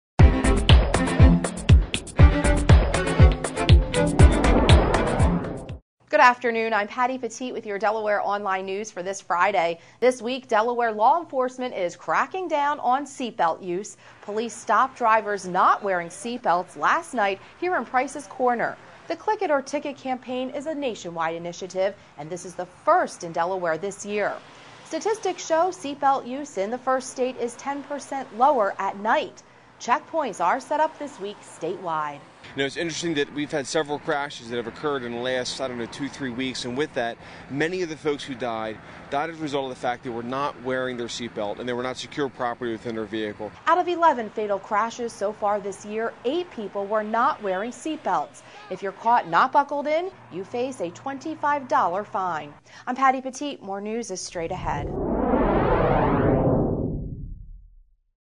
Seatbelt use crack down
Hence the stepped up enforcement of seat belt and child restraint laws on state highways this week, particularly at night, said Andrea Summers, a spokeswoman for the state Office of Highway Safety. She said seat belt use in Delaware is 10 percent lower at night.